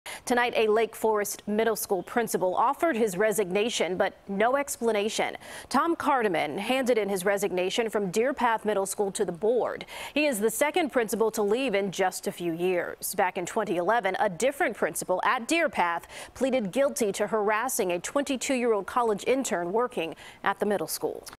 Lake Forest Principal Resigns Without Explanation
Tom Cardamone, principal at Deer Path Middle School, is retiring.